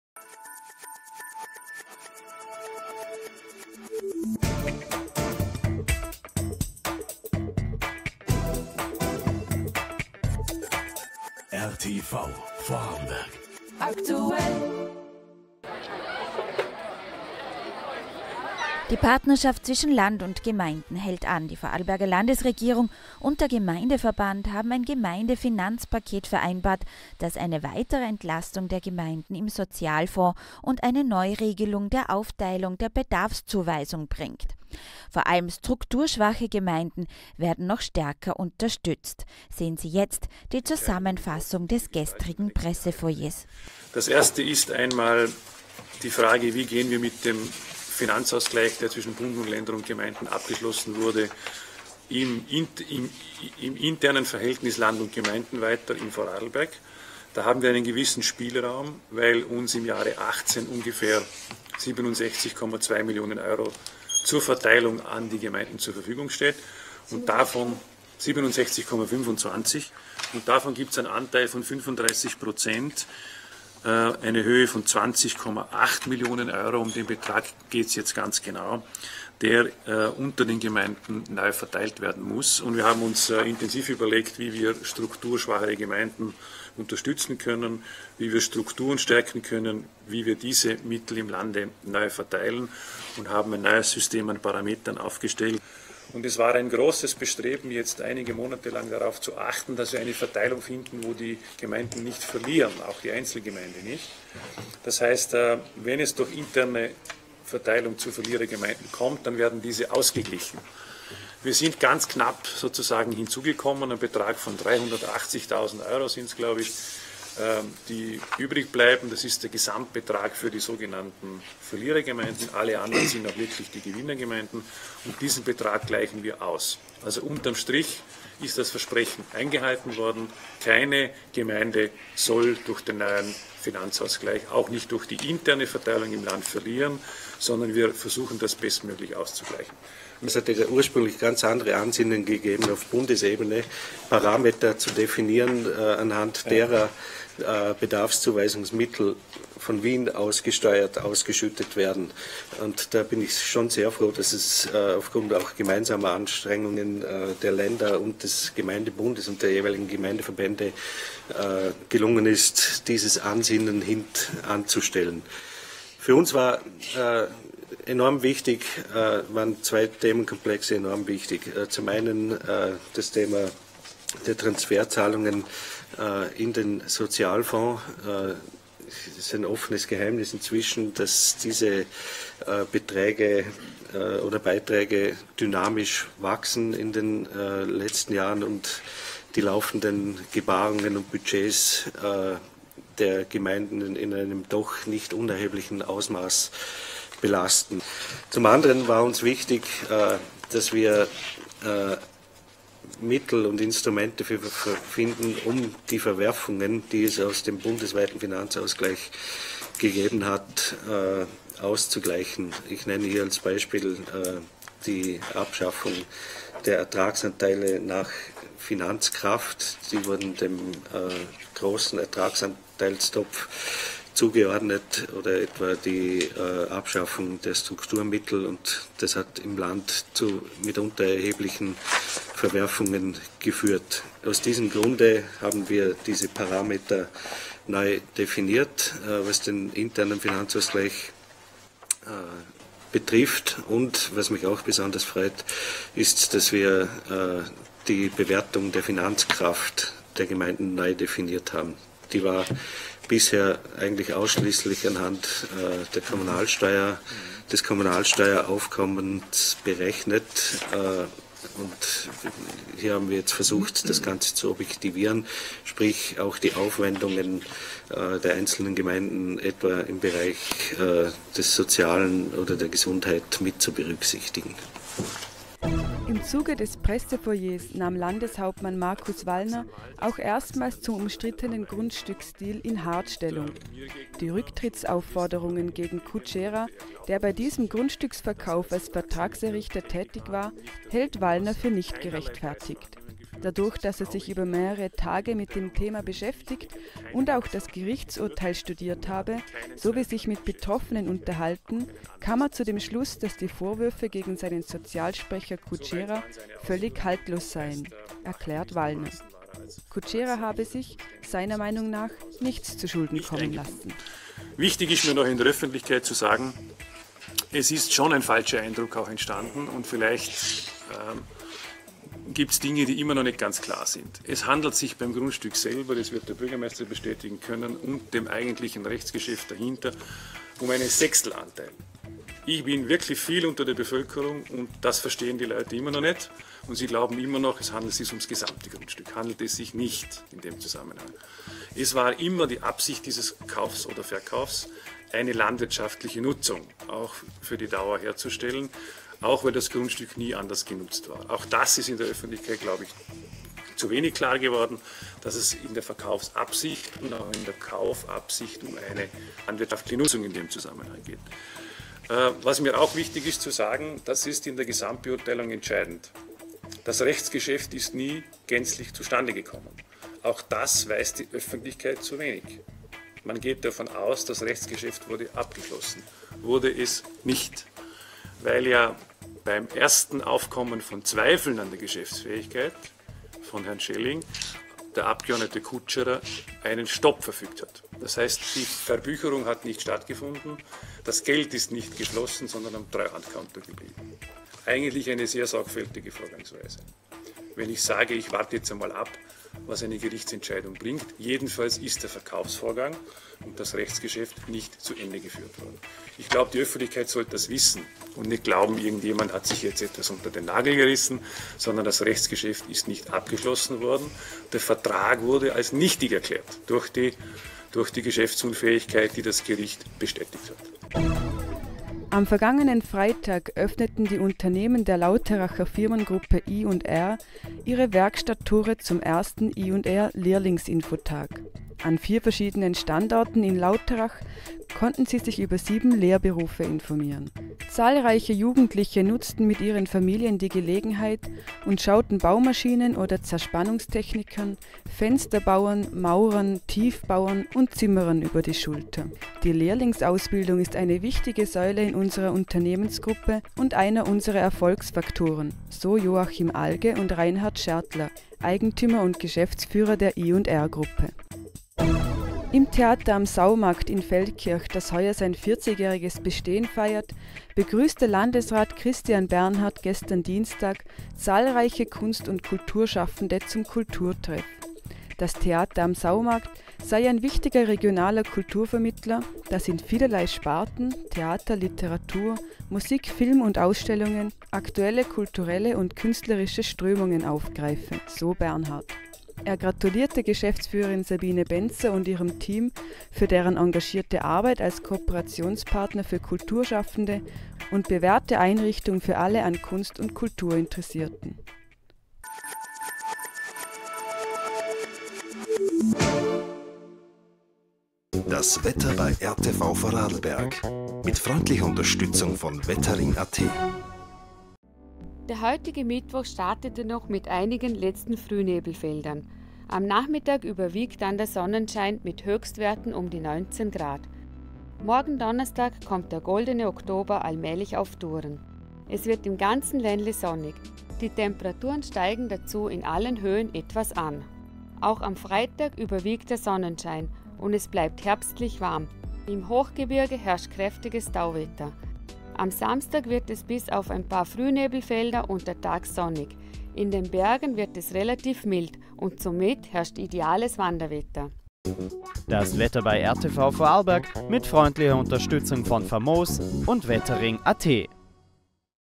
RTV Vorarlberg Aktuell - Pressefoyer Gemeindefinanzierung
Pressefoyer Zusammenfassung zum Thema Gemeindefinanzierung 2017